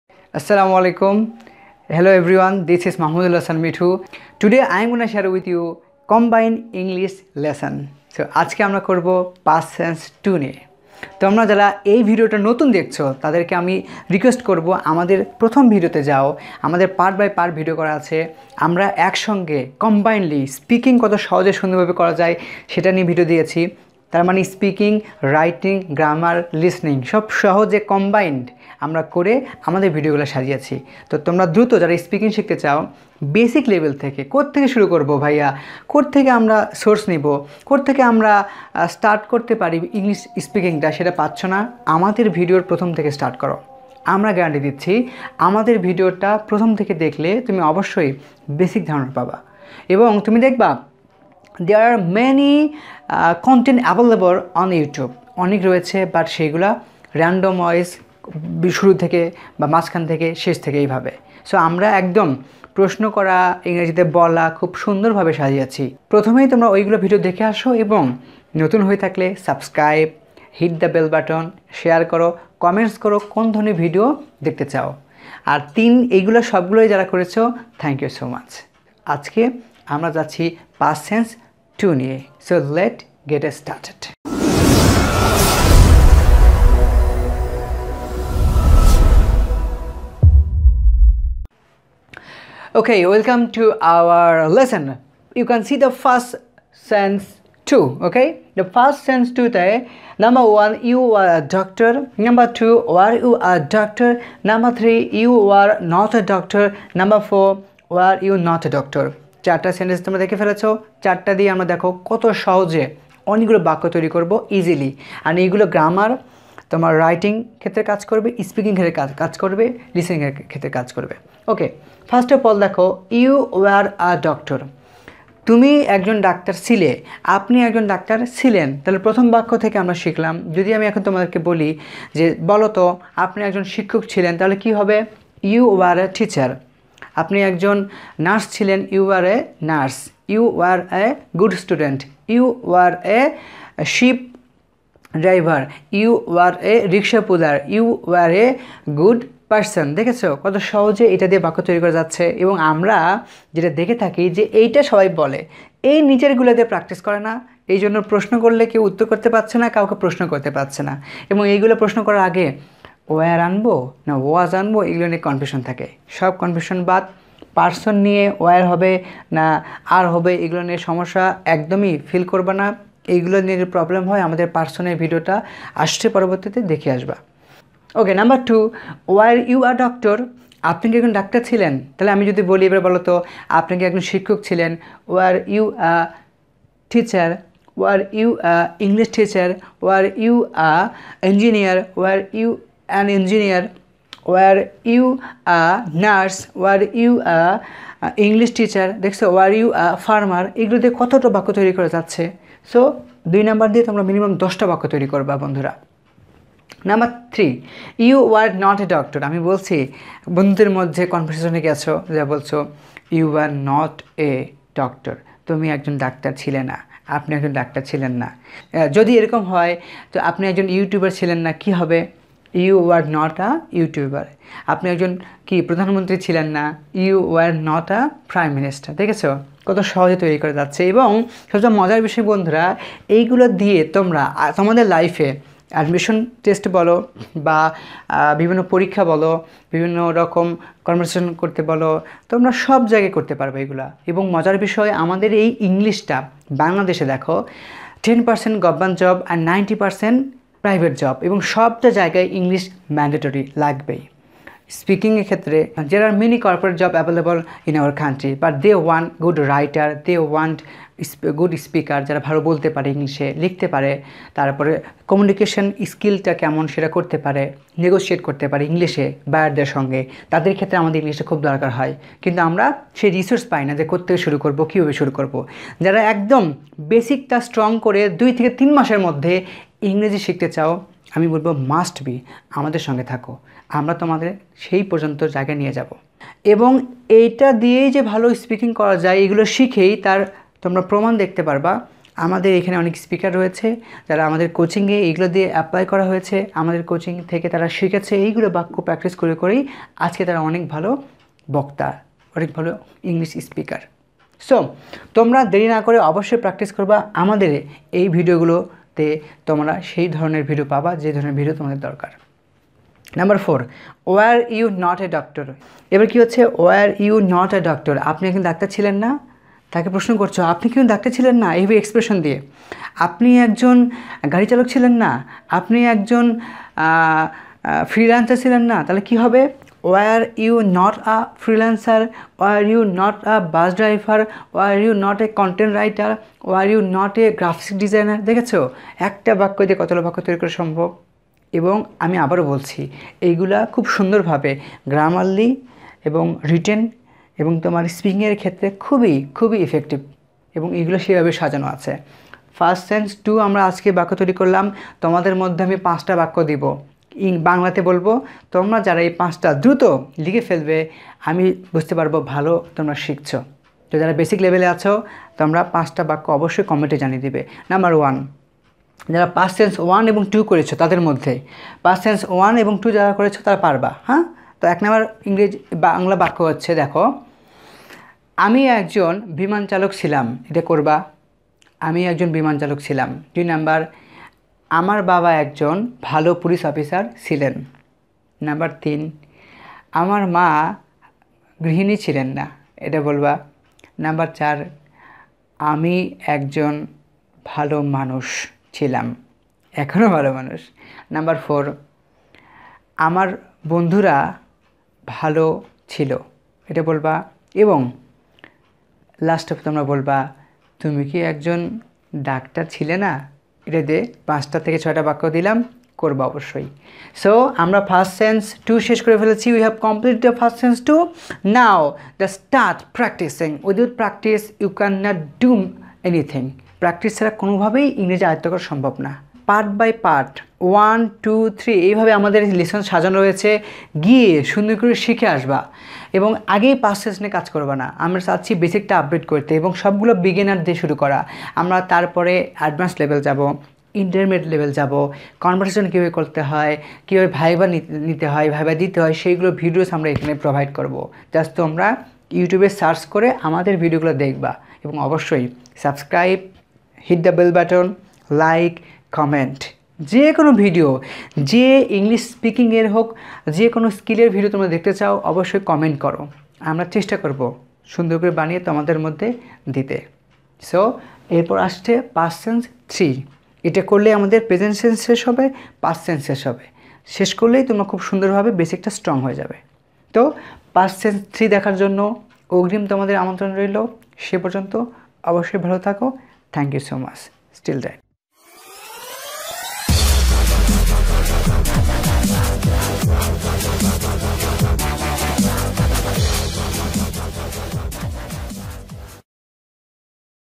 How to start of spoken English in modern way.... Part-5
Hello Friend Here I will show you How to start of spoken English in modern way Past Sense- 2.
1. You were a doctor.
2. Were you a doctor?
3. You were not a doctor.
4. Were you not a doctor?